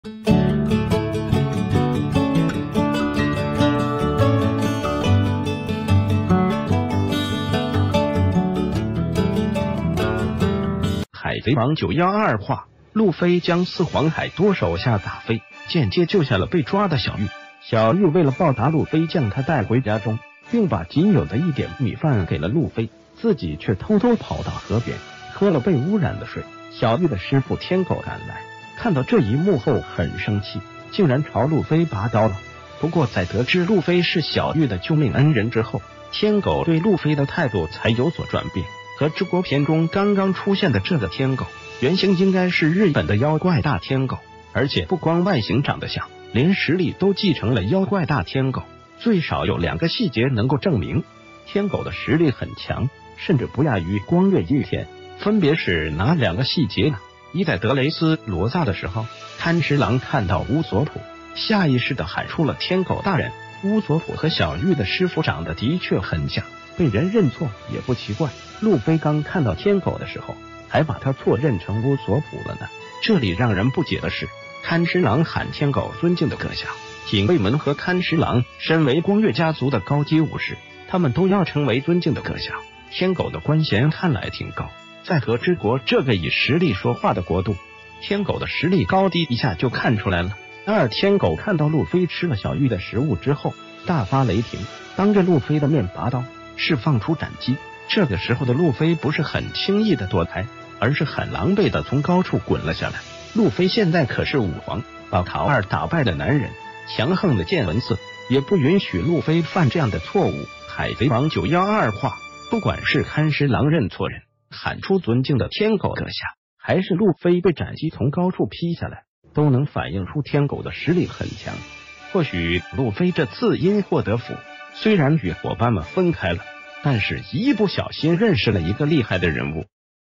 海賊王912話：天狗的實力很強，兩個細節證明不亞于光月御田
海賊王912話，路飛將四皇凱多手下打飛，間接救下了被抓的小玉，小玉為了報答路飛將他帶回家中，并把僅有的一點米飯給了路飛，自己卻偷偷跑到河邊，喝了被污染的水，小玉的師傅天狗趕來，看到這一幕后很生氣，竟然朝路飛拔刀了。

不過在得知路飛是小玉的救命恩人之后，天狗對路飛的態度才有所轉變，和之國篇中剛剛出現的這個天狗，原型應該是日本的妖怪大天狗，而且不光外形長得像，連實力都繼承了妖怪大天狗，最少有兩個細節能夠證明，天狗的實力很強，甚至不亞于光月御田。

分別是哪兩個細節呢：①在德雷斯羅薩的時候，勘十郎看到烏索普，下意識的喊出了“天狗大人”，烏索普和小玉的師傅長的的確很像，被人認錯也不奇怪，路飛剛看到天狗的時候，還把他錯認成烏索普了呢，這里讓人不解的是，勘十郎喊天狗“尊敬的閣下”。錦衛門和勘十郎身為光月家族的高級武士，他們都要稱為“尊敬的閣下”，天狗的官銜看來挺高，在和之國這個以實力說話的國度，天狗的實力高低一下就看出來了。

②天狗看到路飛吃了小玉的食物之后，大發雷霆，當著路飛的面拔刀，釋放出斬擊，這個時候的路飛不是很輕易的躲開，而是很狼狽的從高處滾了下來，路飛現在可是五皇，把卡二打敗的男人，強橫的見聞色也不允許路飛犯這樣的錯誤。

海賊王912話，不管是勘十郎認錯人，喊出“尊敬的天狗閣下”，還是路飛被斬擊從高處劈下來，都能反映出天狗的實力很強，或許路飛這次因禍得福，雖然與伙伴們分開了，但是一不小心認識了一個厲害的人物。